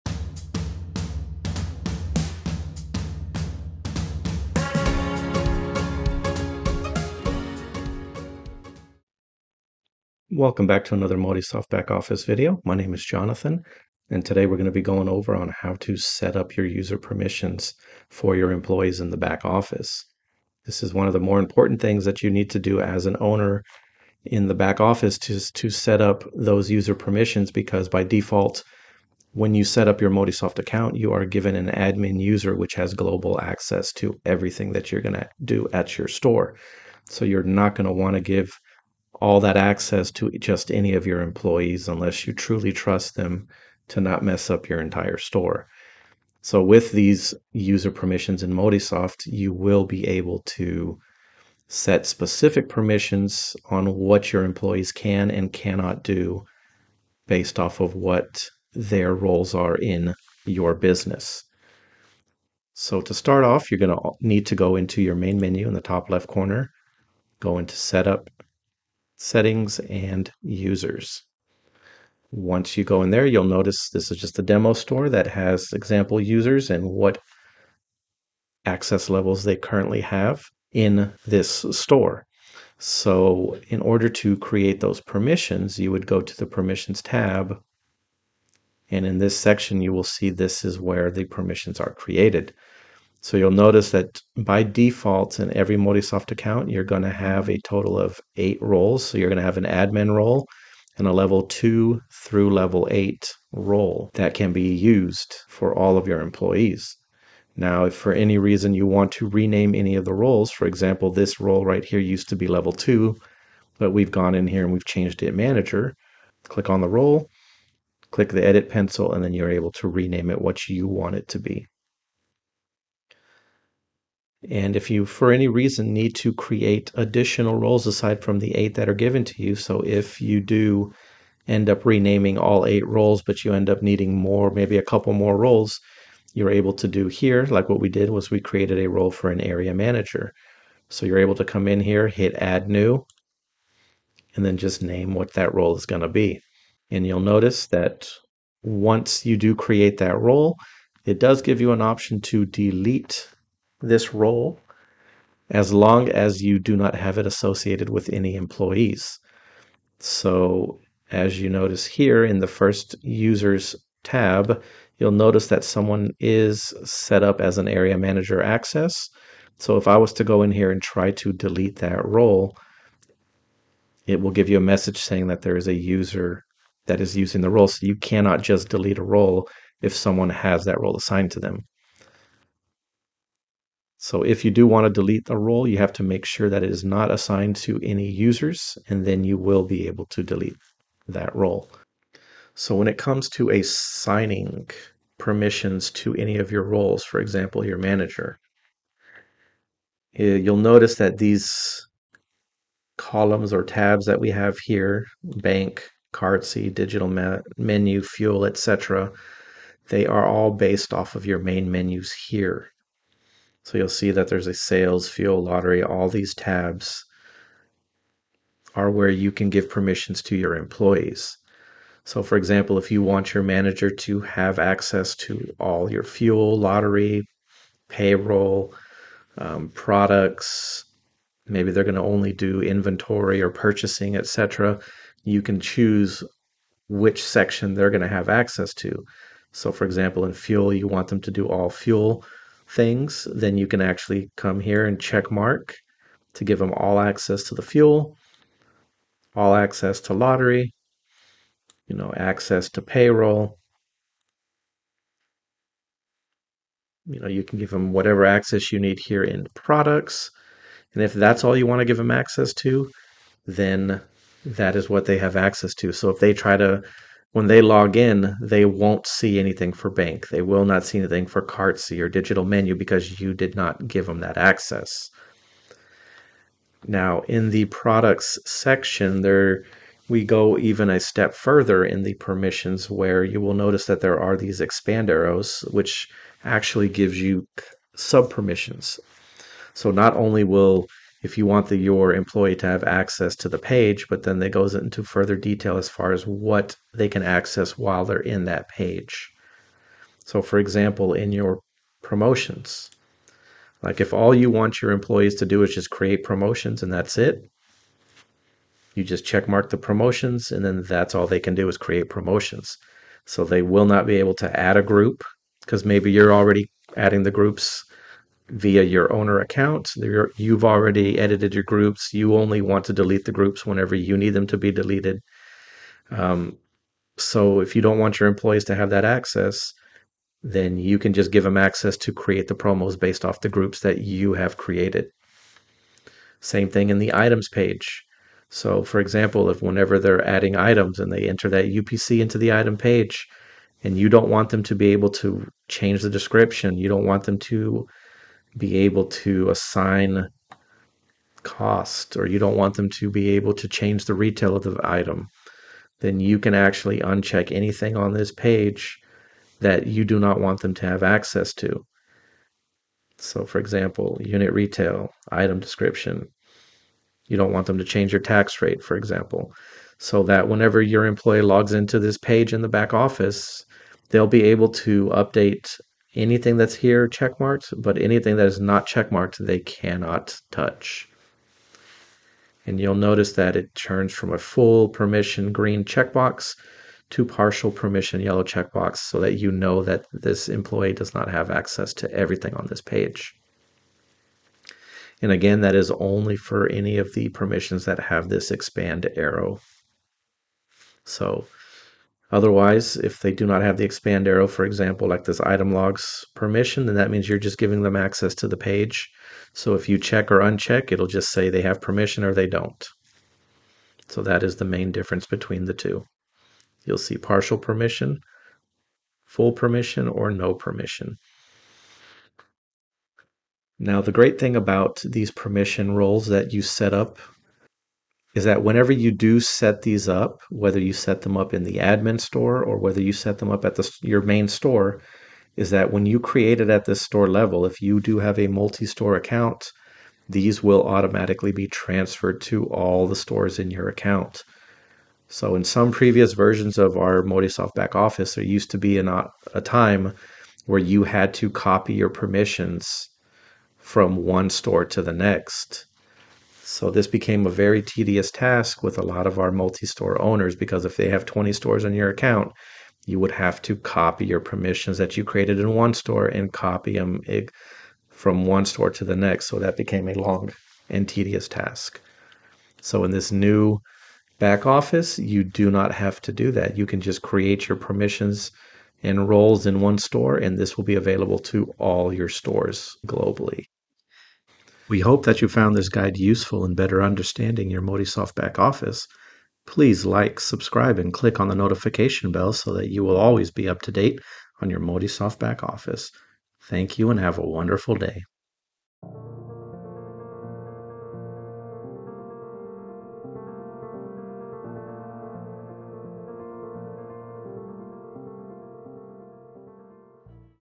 Updating User Permissions
Learn how to set your user permissions so your employees will not have access to everything in the back office under the Owner account profile.  When you create a new user for an employee, you can give them access to only what you want them to see. Create time restrictions and IP restrictions as well for more security.